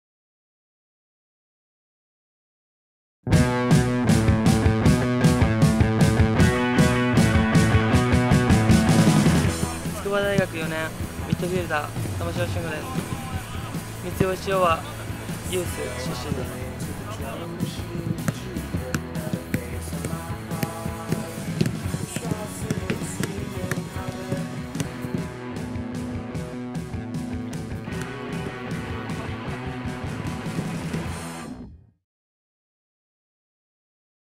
【CSParkSkill】リフティングテクニック 玉城 峻吾（筑波大学蹴球部４年）
大学サッカー選手に華麗なるリフティングテクニックを披露してもらいました！

筑波大学蹴球部４年
三菱養和ユース
MF 玉城 峻吾